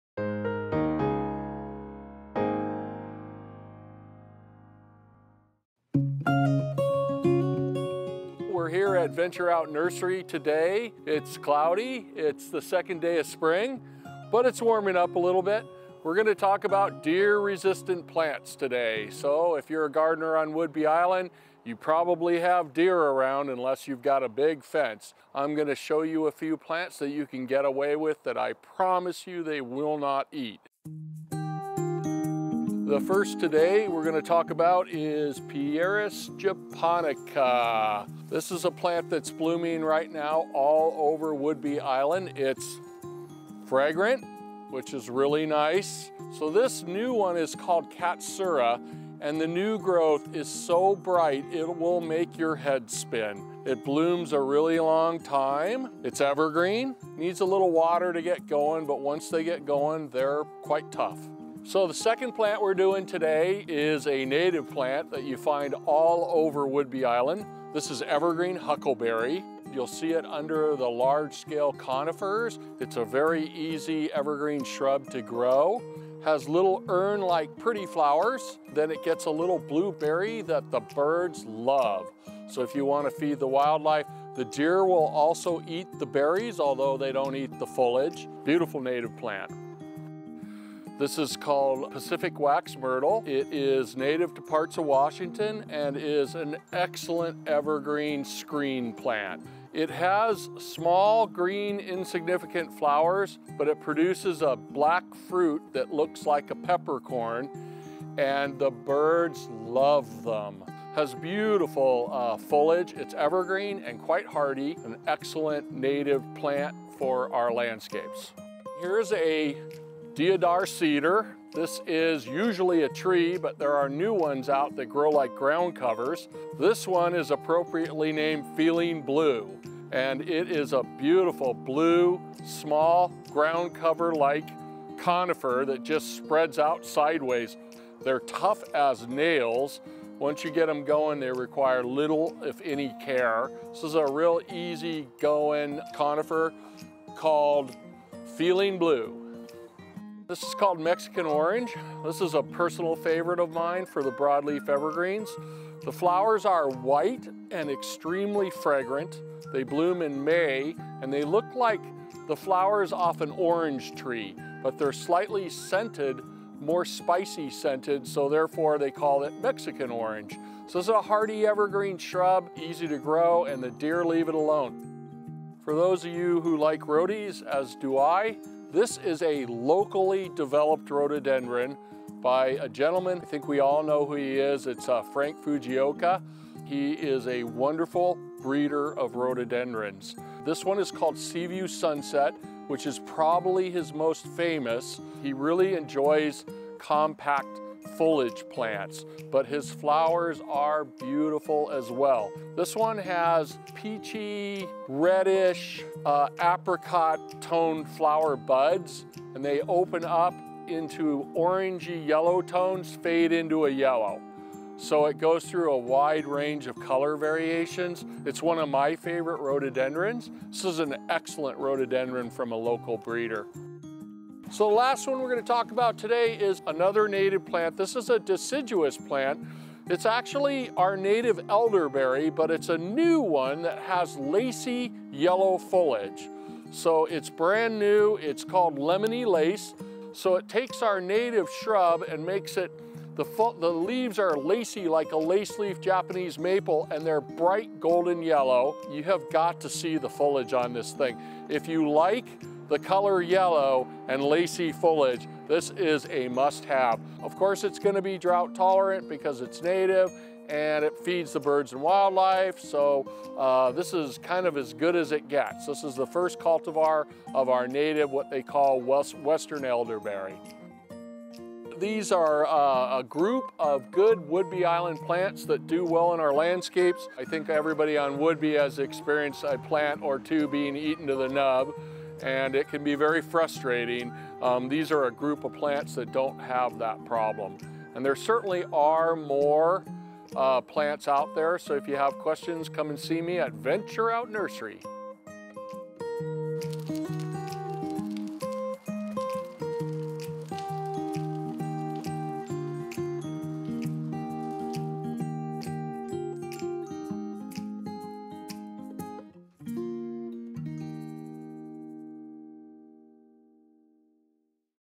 Deer Resistant Plants with Venture Out Nursery
Deer eating your plants? Local gardening expert Eric Studebaker guides you through a variety of deer resistant plants that will add some character to your lawn and are easy to maintain.